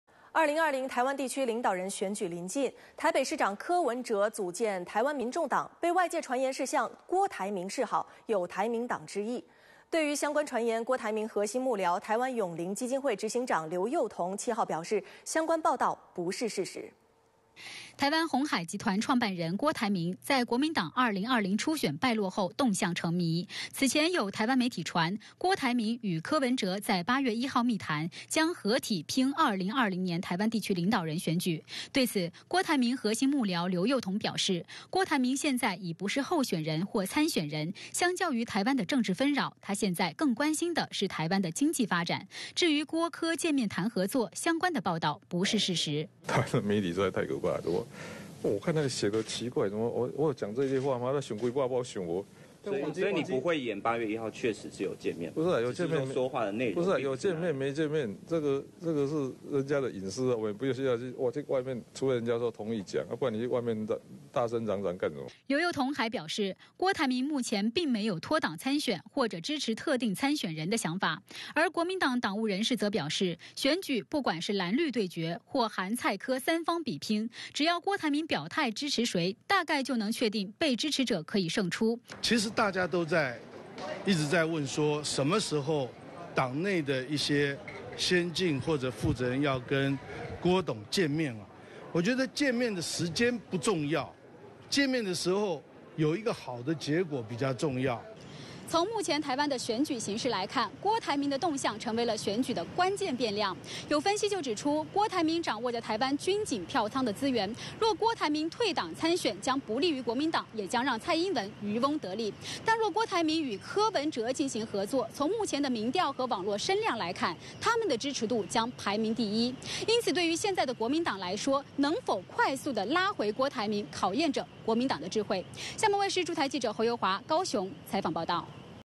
“郭柯配”已成形？郭台銘幕僚：不是事實！
2020台灣地區領導人選舉將近，台北市長柯文哲組建"台灣民眾黨"被外界傳言是向郭台銘示好，有"台銘黨"之意。對於相關傳言，郭台銘核心幕僚、台灣永齡基金會執行長劉宥彤表示：相關報導不是事實．．．